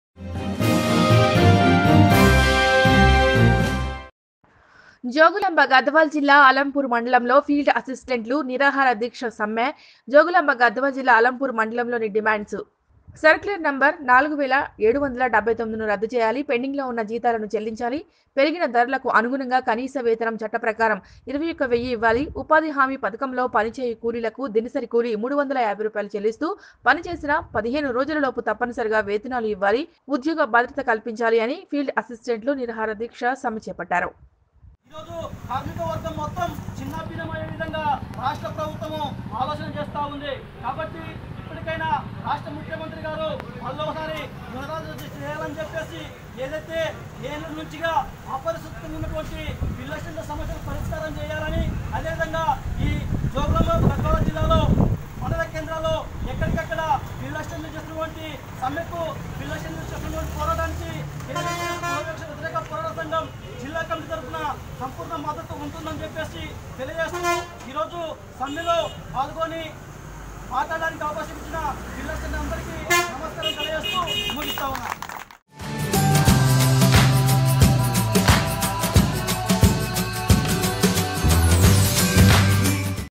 జోగులాంబ గద్వాల జిల్లా అల్లంపూర్ మండలంలో ఫీల్డ్ ఆసిస్టెంట్లు నిరాహారదీక్ష సమ్మె || Nethra News
|| Nethra News || Jogulamba Gadwal ||